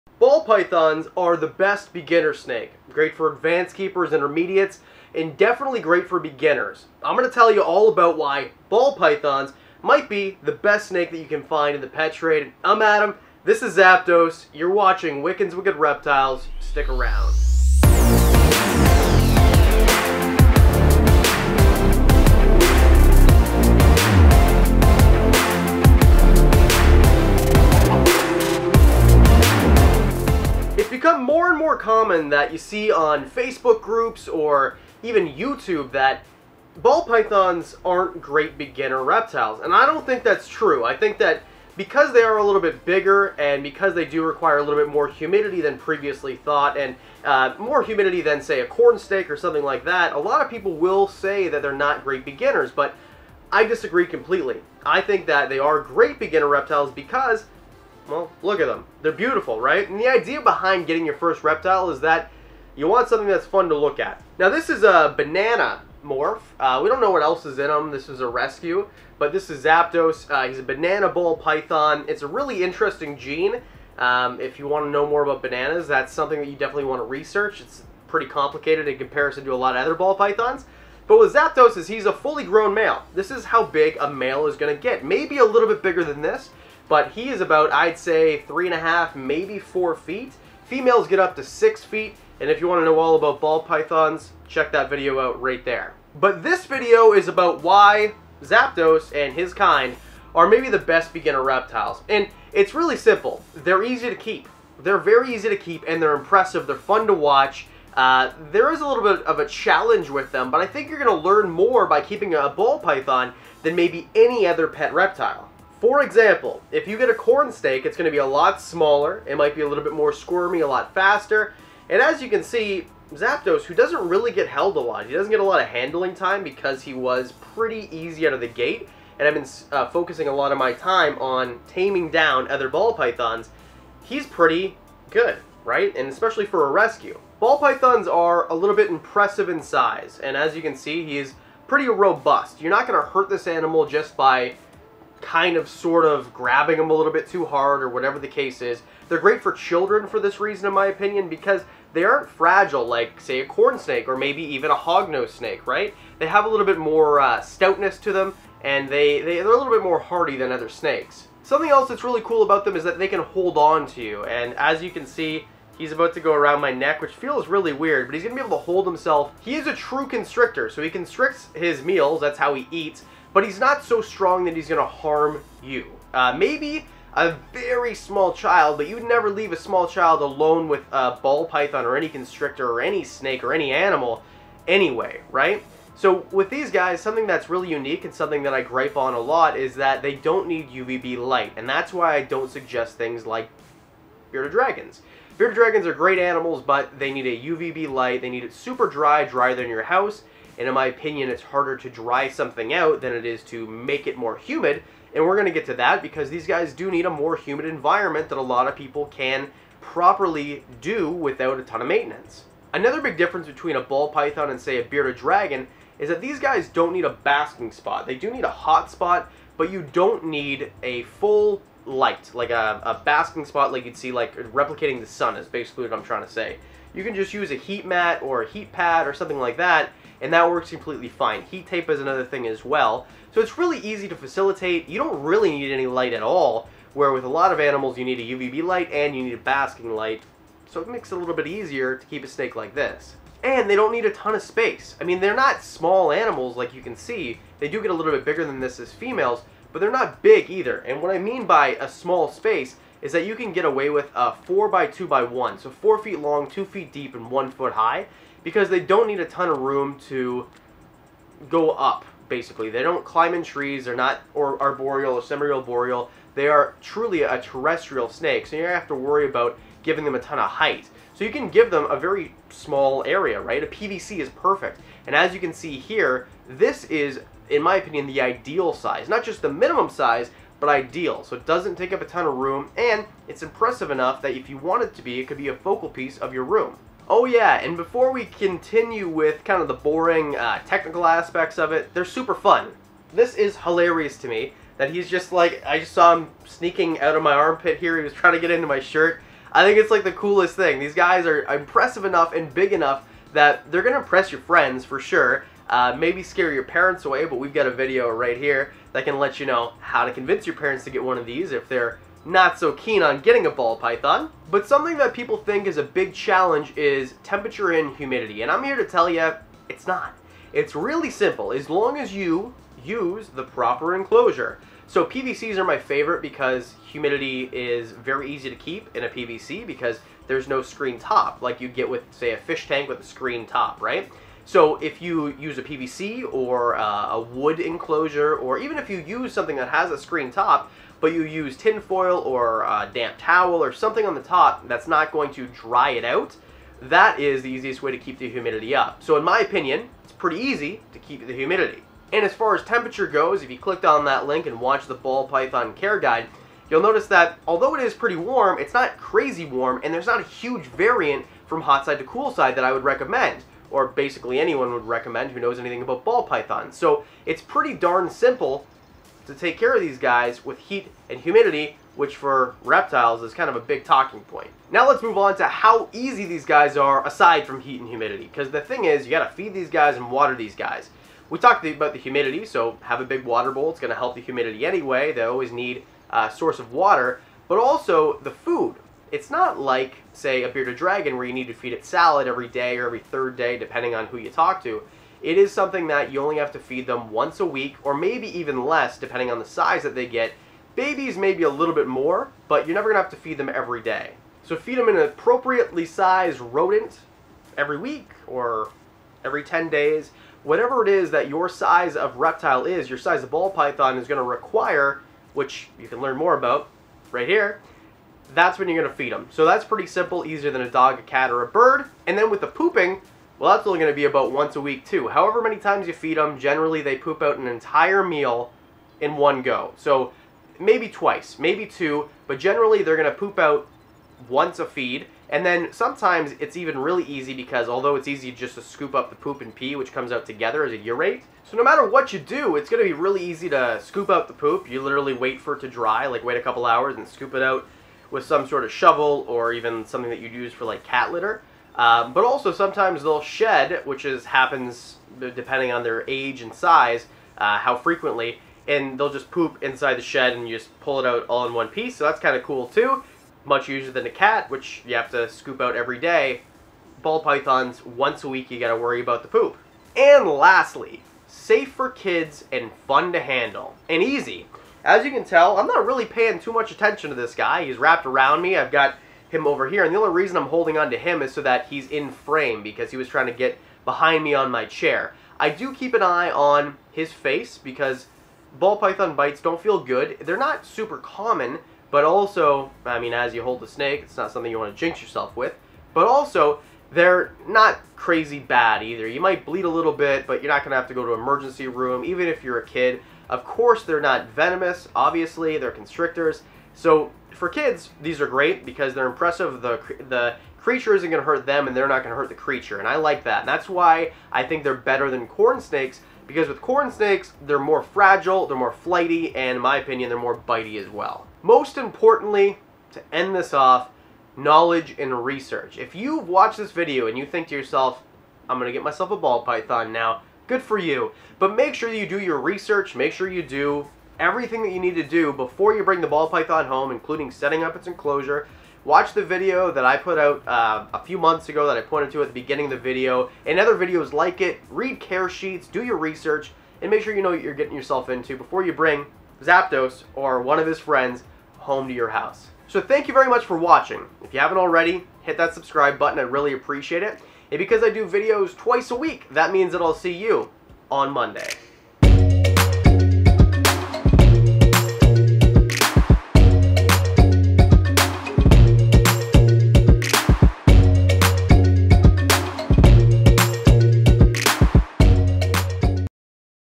Ball Pythons Are The Perfect Pet Reptile
Ball Pythons might be the perfect pet reptile for beginners and beyond! Sometimes these beautiful creatures get labeled as difficult to keep and this week I wanted to outline why they are maybe the best pet reptile you can own.